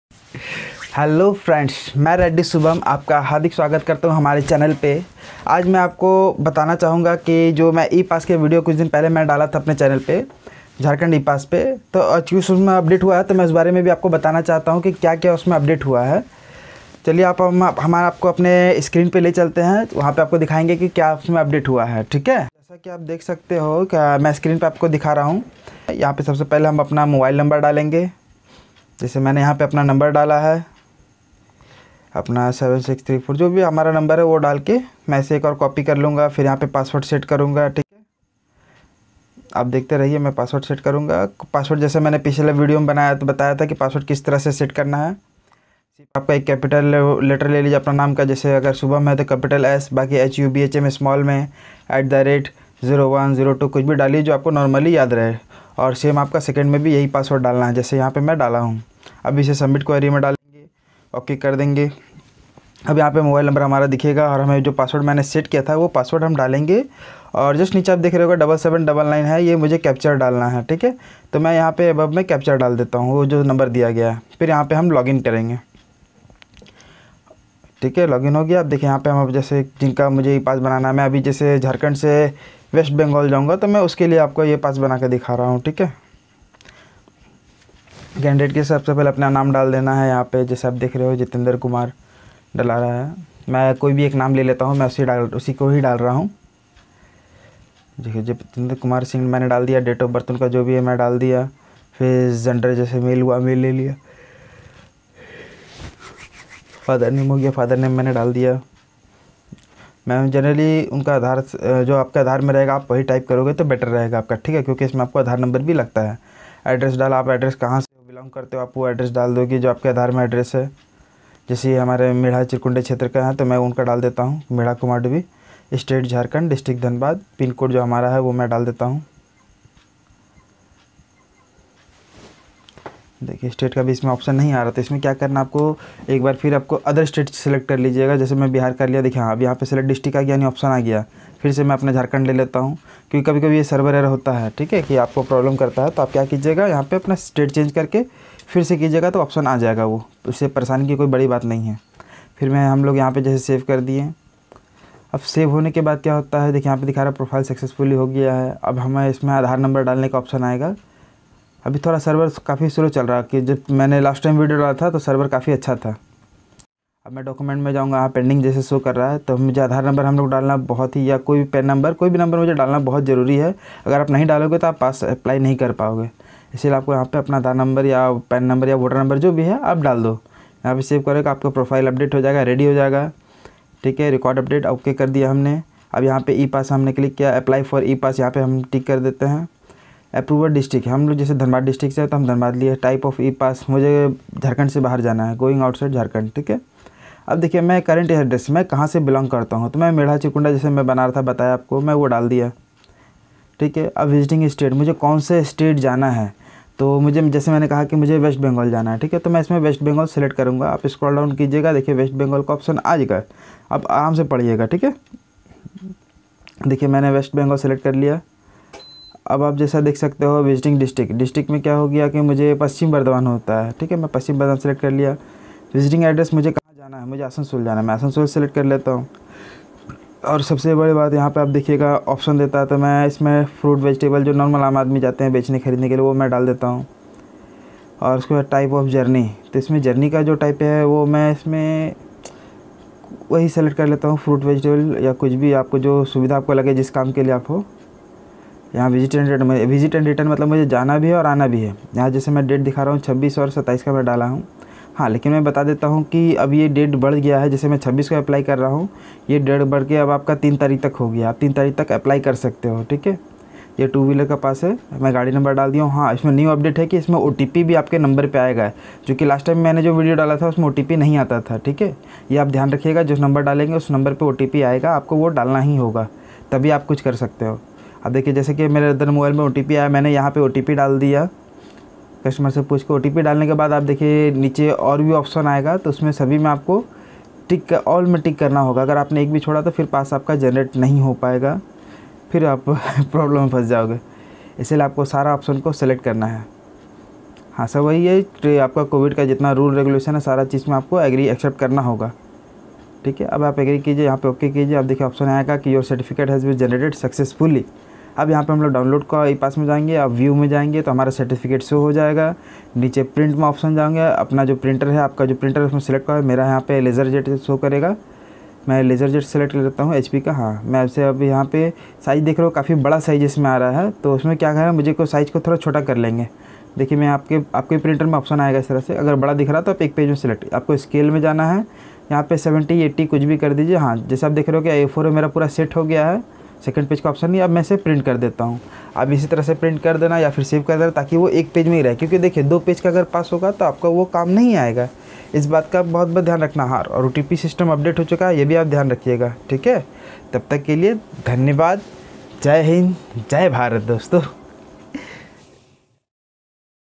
JHARKHAND E - PASS LATEST UP DATE , E PASS JHARKHAND, HOW TO APPLY E PASS
HELLO FRNDS kese ho app
 pls app log safe rahena . jarurat padne par hi apne gharo se bahar nikalna
ajj me apne is video me jharkhand e pass me jo up date hua hai usaka bare me bataya hu . new up date me App jo Mobile no se register karo ge uss par
OTP bhi jayega. with out OTP app nahi banaa paoge apna e pass
pls app Video pura dekhna to bahot asani sesamajh jaoge .
thanku my dear frnds
app ka dost redi shubham